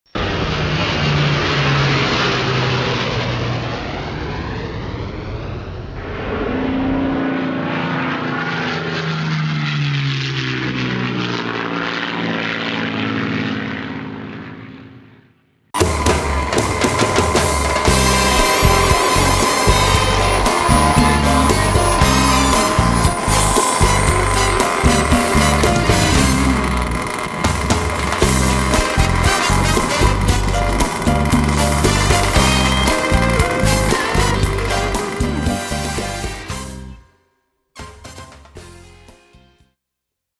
R/C Turbine Powert Jet El Storm Jet Short Video
Storm jet landing at Mako's Jet Club